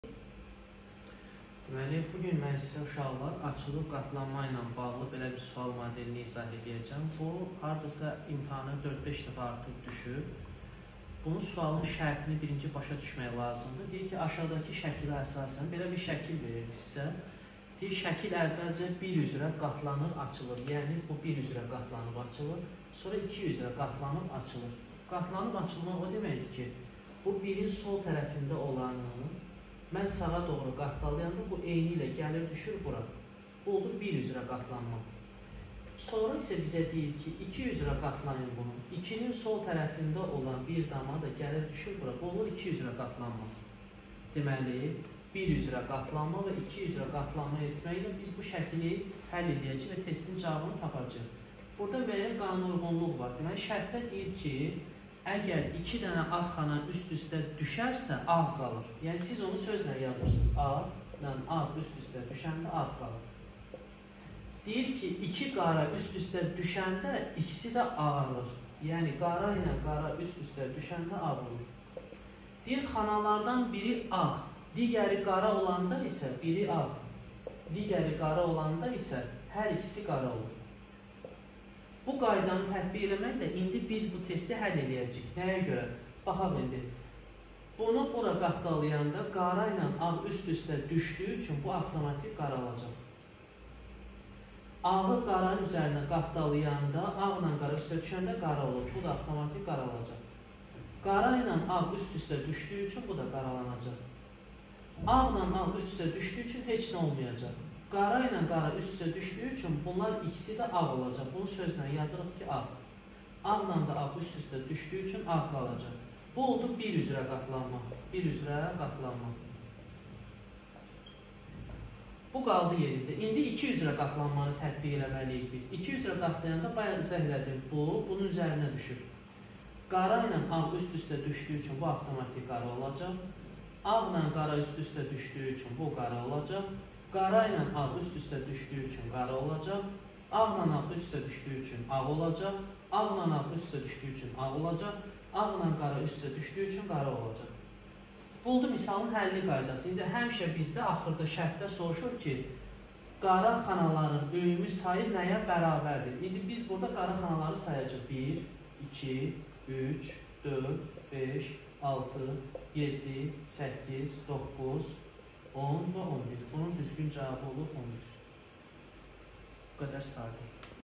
Açılıb qatlanma sualının izahı | Məntiq | Dövlət qulluğu |Turan Mammadli team
➡️Açılıb-qatlanma ilə bağlı sualın izahı.

🤓Bu sualı çox tələbə səhv yazır. Əslində çox asan izahı var.

Müəllim: Tural Ağayev (Məntiq/İnformatik)

📍İçəri şəhər, Asəf Zeynallı k. 31/33
(İsveçrə səfirliyinin arxası)